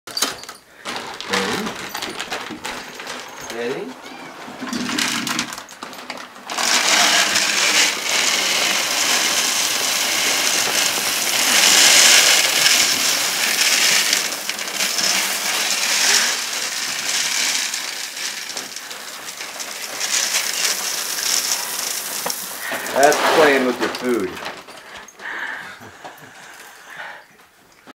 Ultimate Cat Tease: funny Cats feeding frenzy
The Cat Food Bag Shows Up once a month and it's a big deal around these parts..!!
Without hesitation, the cats dig right into their new food.
Eating while the food buries the cats alive in goodness.